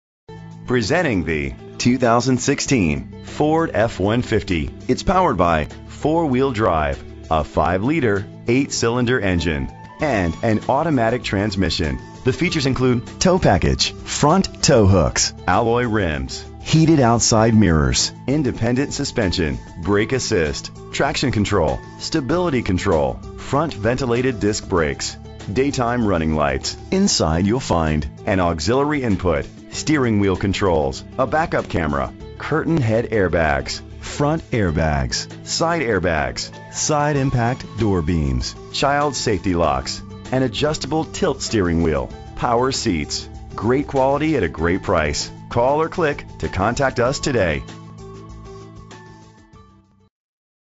2016 Ford F-150 T35071-1 - Roanoke VA
Year: 2016
Make: Ford
Model: F-150
Trim: XLT
Engine: 5.0 liter 8 Cylindercylinder 4WD
Transmission: Automatic
Color: White
Mileage: 1
Address:
809 Williamson Rd.
Roanoke, VA 24016
VIN:1FTFX1EFXGFC37711